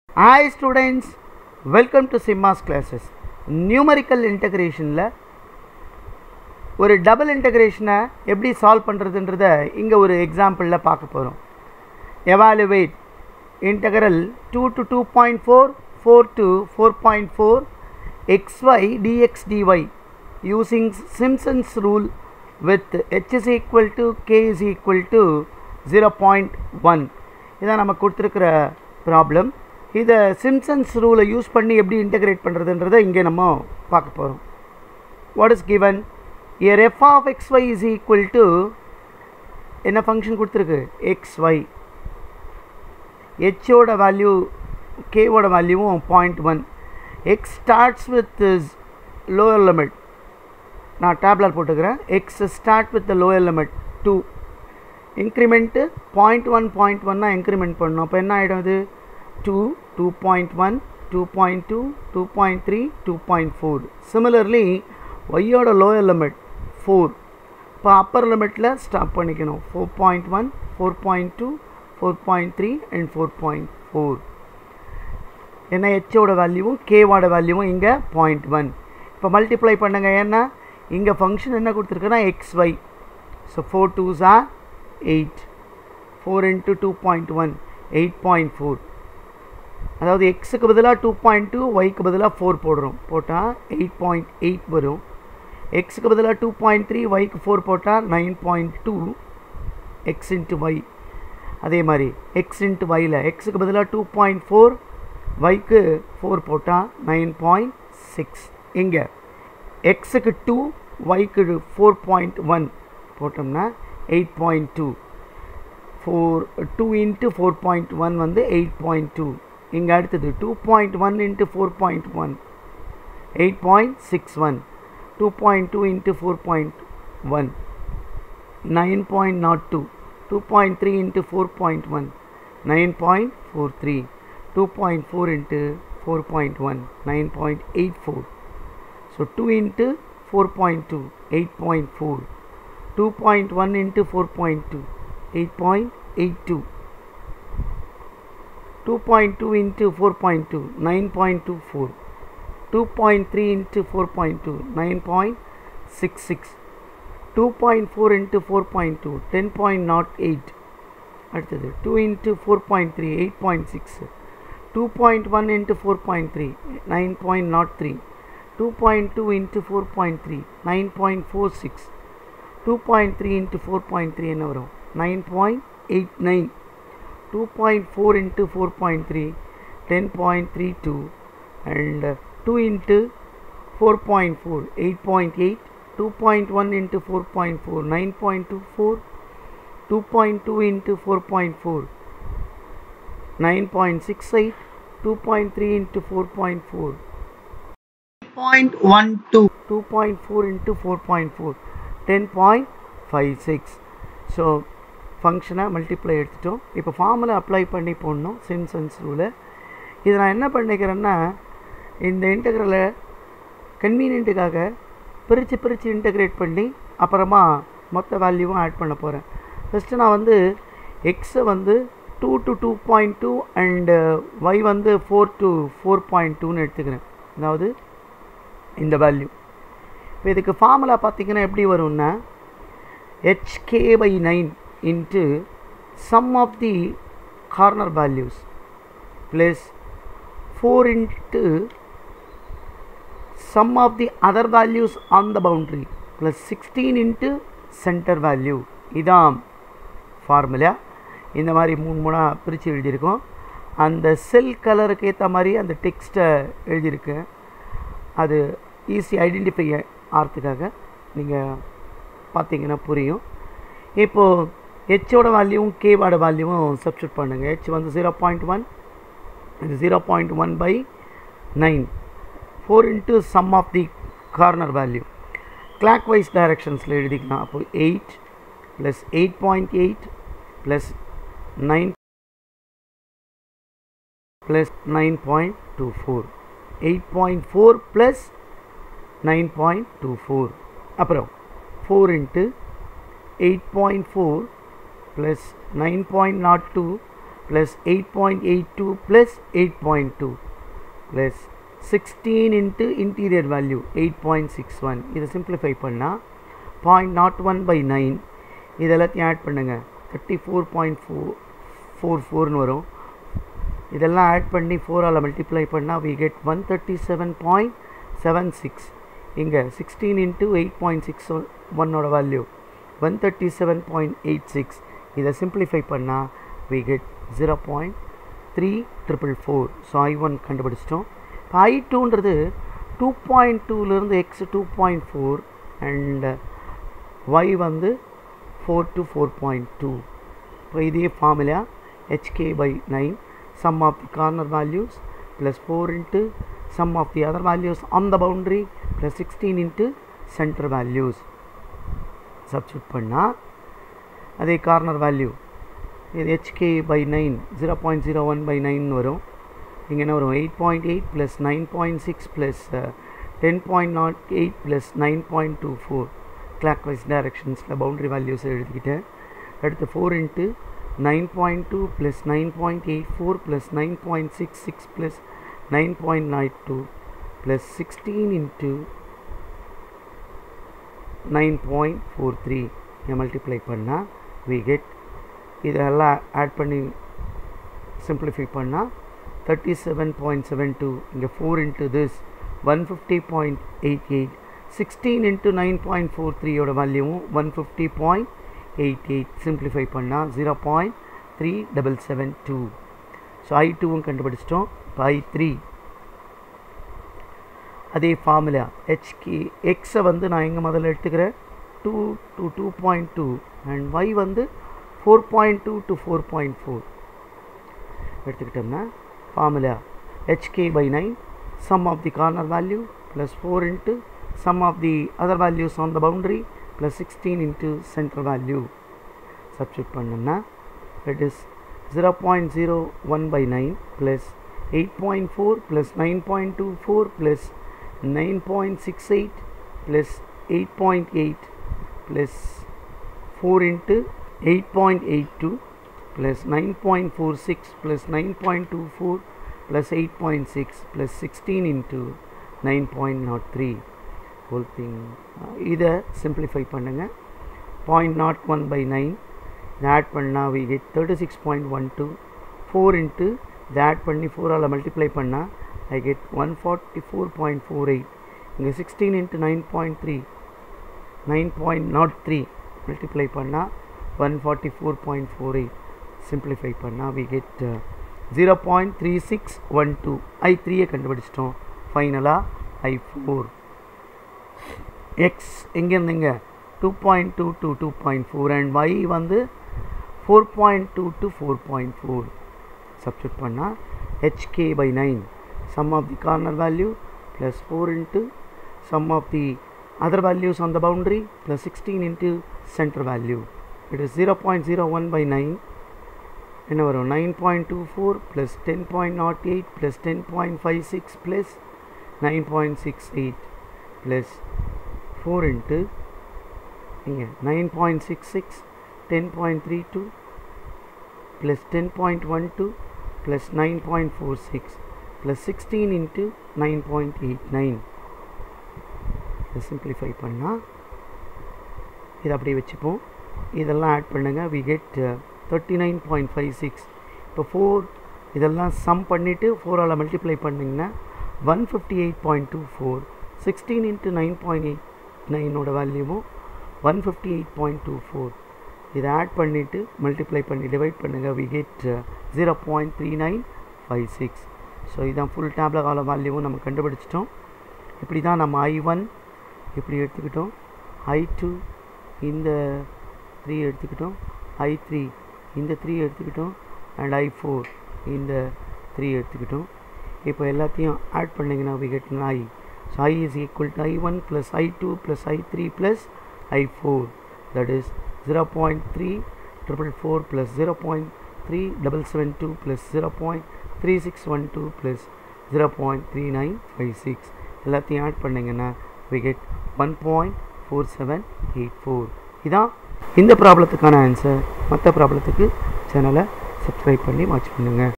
Simpson Evaluate integral 2 to 2.4 integral 4 to 4.4 xy dx dy Using Simpson's rule with h=k=0.1
Evaluate double integral 0 to 0.5   sin{xy} by (1+xy)  dx dy Using Simpson's rule with h=k=0.25